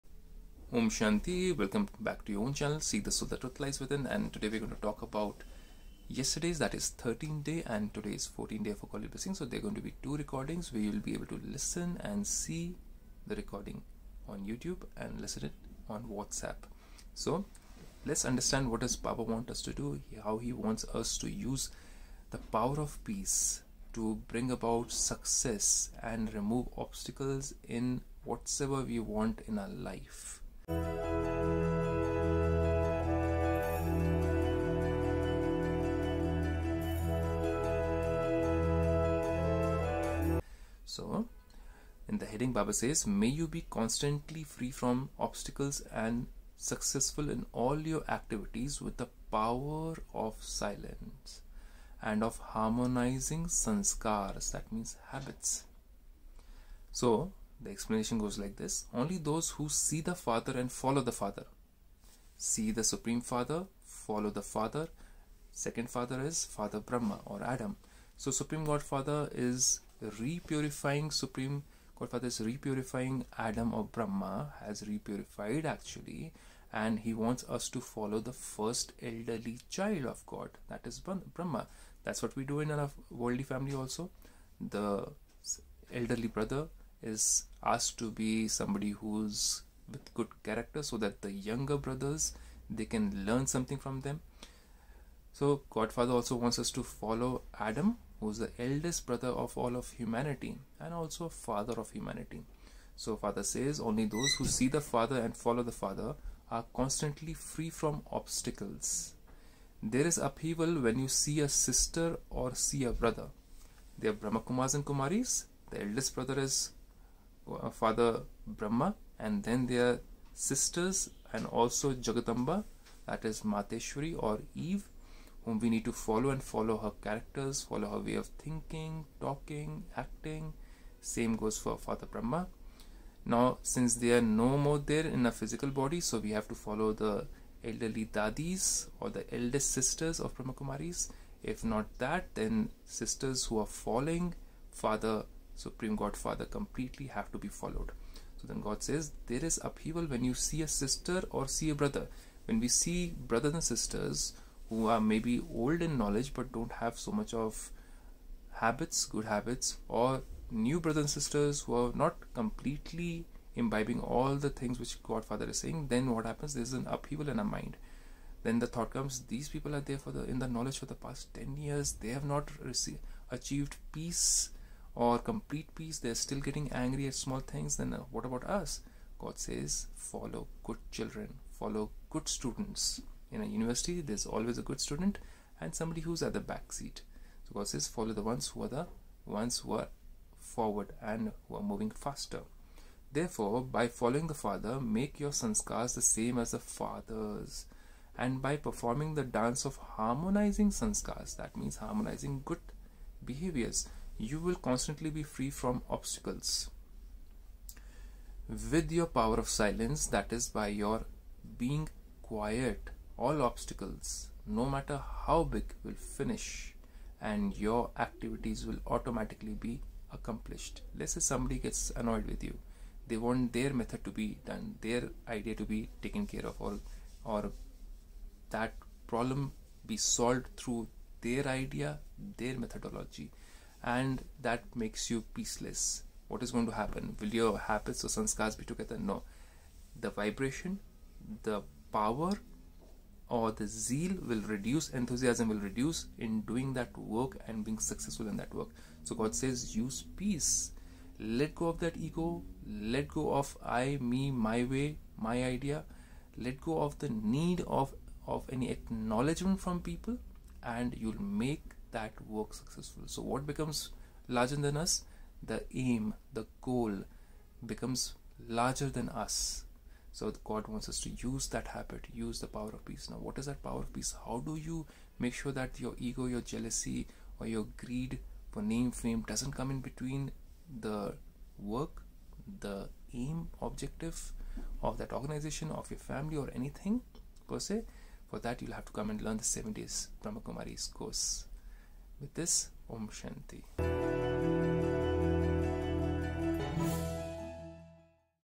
With the power of silence we can be obstacle free & successful.
DAY 13 Whenever we use the power of silence , it helps us be in-tune with people around who matter for any task to be completed . It brings about harmony & thus makes us obstacle free . For this , it is important to follow supreme father and not anybody else .

Om Shanti .